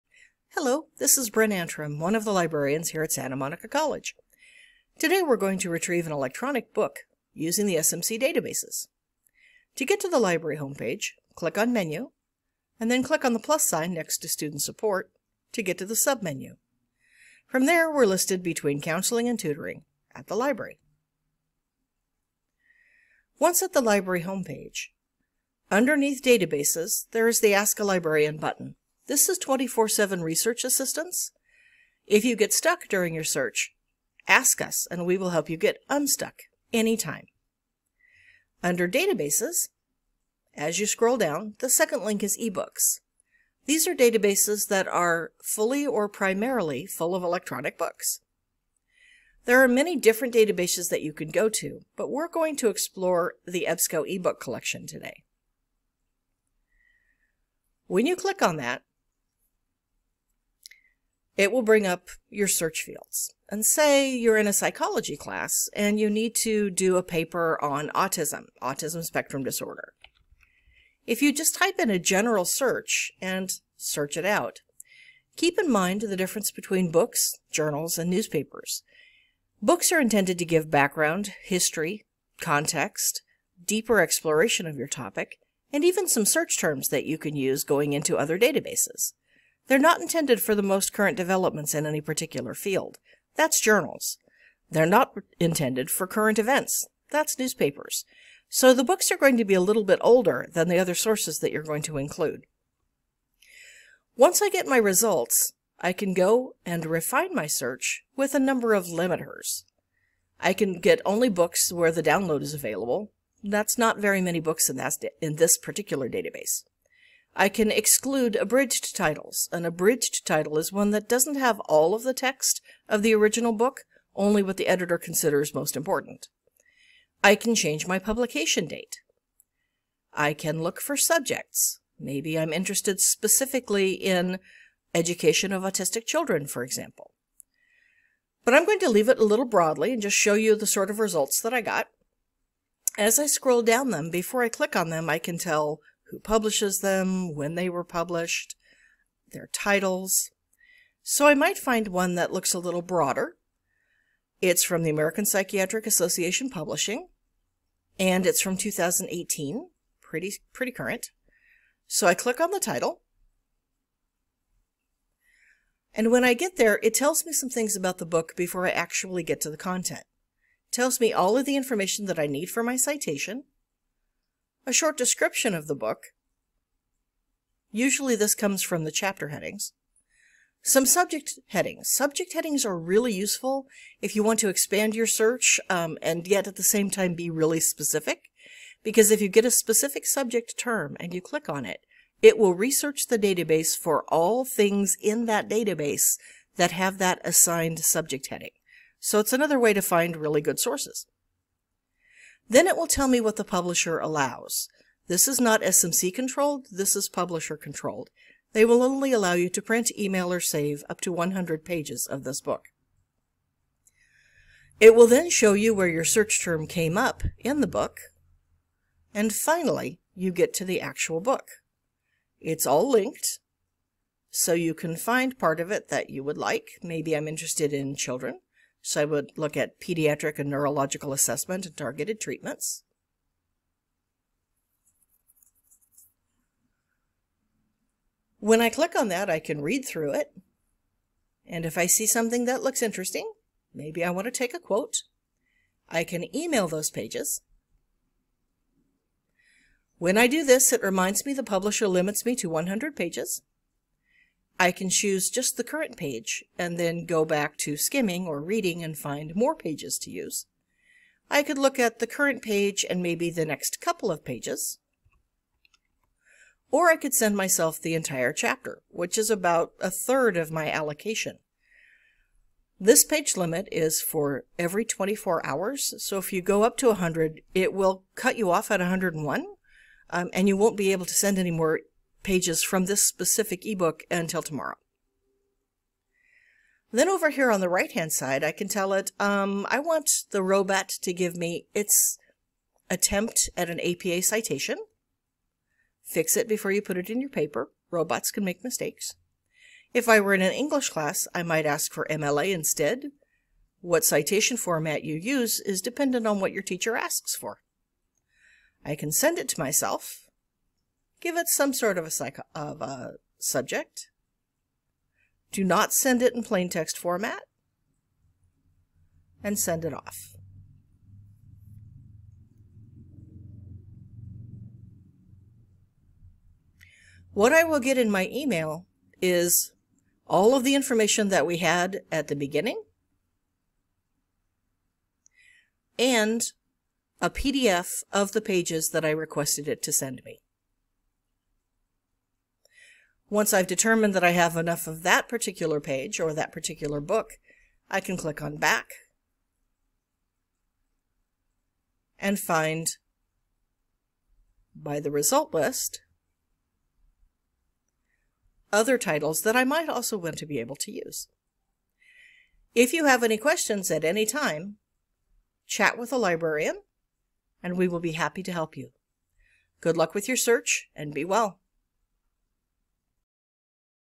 Ebsco Ebooks Collection
A short exploration of how to find electronic books in the Ebsco Ebooks Collection at Santa Monica College.